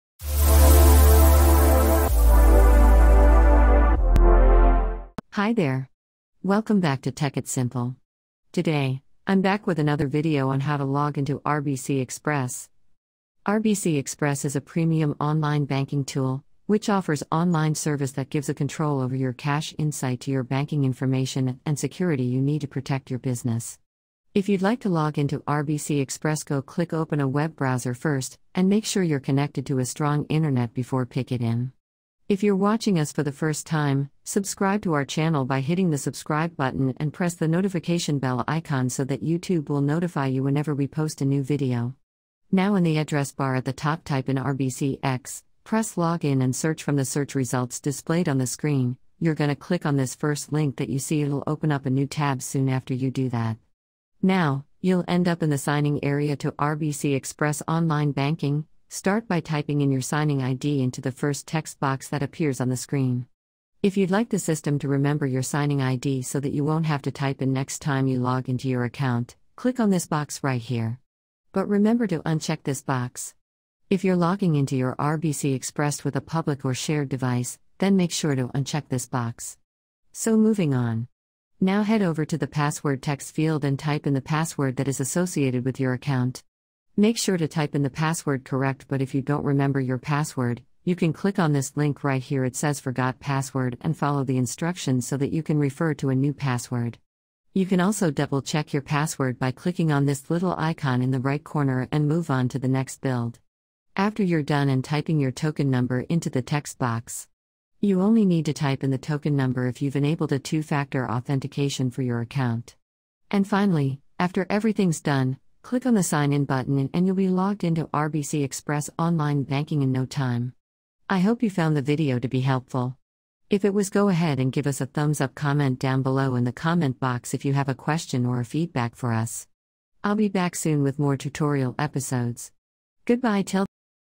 How To Login RBC Express Bank? Watch Step By Step To Sign In RBC Express Bank Tutorial Video
Hey Everyone. In Today's Video I Will Show You How To Login RBC Express Bank. Watch Step By Step To Sign In RBC Express Tutorial Video.